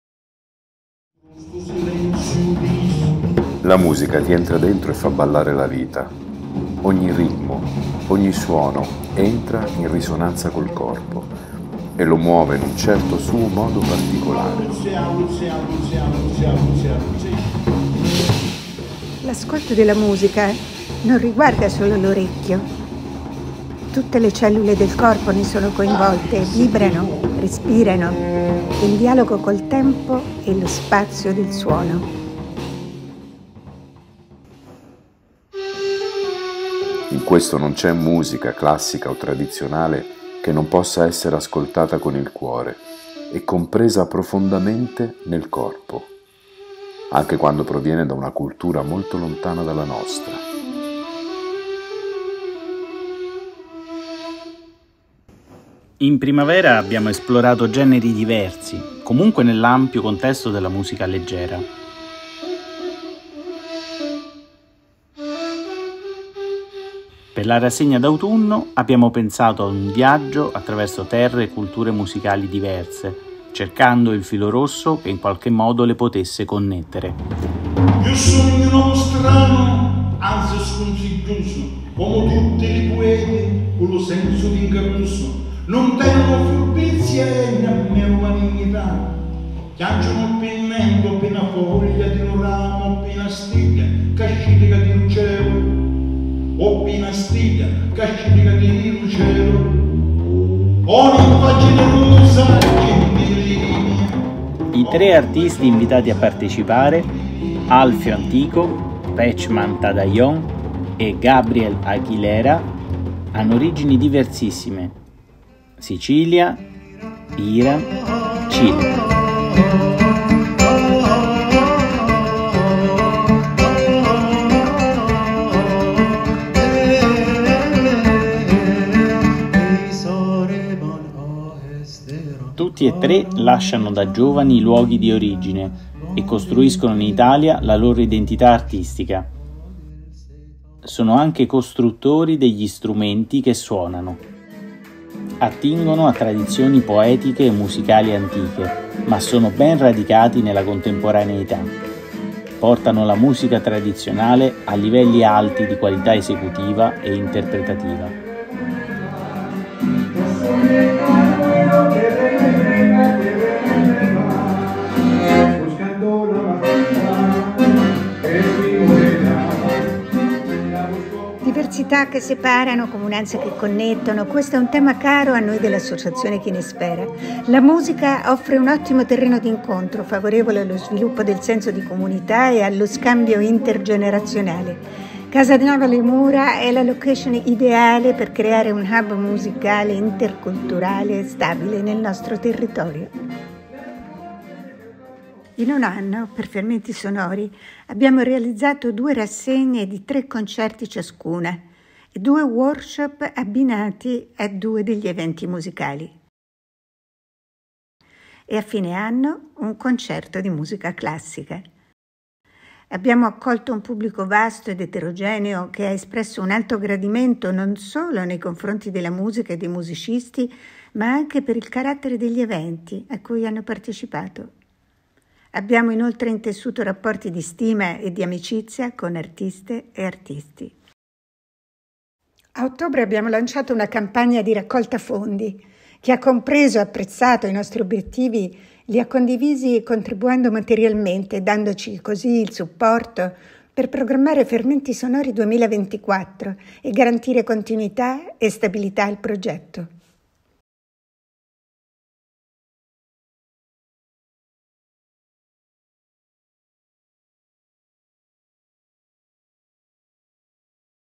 Fermenti Sonori Crowdfunding Last Call
Fermenti Sonori 2023 (English version below). Un anno di attività musicale a CasaNave Alle Mura, Tuscania (VT). Due rassegne di tre concerti ciascuna, due workshop abbinati a due degli eventi musicali, un concerto di musica classica a fine anno.

Kinesfera ringrazia gli artisti che hanno collaborato: Clara Graziano, Teresa Spagnuolo, Pina Valentino della Banda della Ricetta; Arturo Stàlteri; Francesco Attesti, Maicol Cucchi, Mauro Maurizi di AB&C Trio; Alfio Antico e Amedeo Ronga; Pejman Tadayon con Elena Mozzetta; Gabriel Aguilera, Fiore Benigni, Karen Gómez del Trio EnCanto; Tullio Forlenza.

Grazie a tutto il pubblico che ha seguito e partecipato attivamente.
Grazie al piccolo team di Kinesfera e a chi a generosamente collaborato in alcuni eventi.

Grazie a Jeanette Vuocolo, Lucrezia Testa Iannilli e Marco Guidi per le immagini foto e video.

Infine grazie e chi ha sostenuto Fermenti Sonori contribuendo alla raccolta fondi sulla piattaforma C'è ancora tempo fino alla mezzanotte del 7 gennaio per diventare sostenitori di Fermenti Sonori 2024 con una donazione di qualsiasi entità!

Fermenti Sonori 2023. A year of musical activity at CasaNave Alle Mura, Tuscania (VT). Two festivals of three concerts each, two workshops combined with two of the musical events, a classical music concert at the end of the year.

Kinesfera would like to thank the artists: Clara Graziano, Teresa Spagnuolo, Pina Valentino of Banda della Ricetta; Arturo Stàlteri; Francesco Attesti, Maicol Cucchi, Mauro Maurizi of AB&C Trio; Alfio Antico and Amedeo Ronga; Pejman Tadayon with Elena Mozzetta; Gabriel Aguilera, Fiore Benigni, Karen Gómez of Trio EnCanto; and Tullio Forlenza.

Thanks to all the audience who followed and actively participated.
Thanks to the Kinesfera's Little Team and those who generously collaborated in some events.

Thanks to Jeanette Vuocolo, Lucrezia Testa Iannilli and Marco Guidi photo and video.

Finally thanks and those who supported Fermenti Sonori by contributing to the fundraising on the platform There is still time until midnight on January 7 to become a supporter of Fermenti Sonori 2024 with a donation of any size!